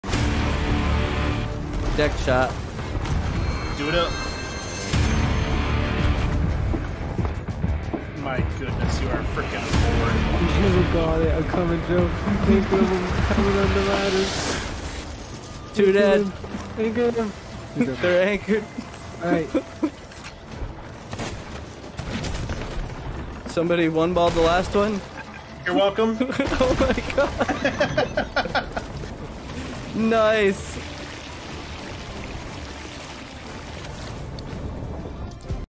8 ball, corner pocket 😉
Calling out a deckshot and nailing it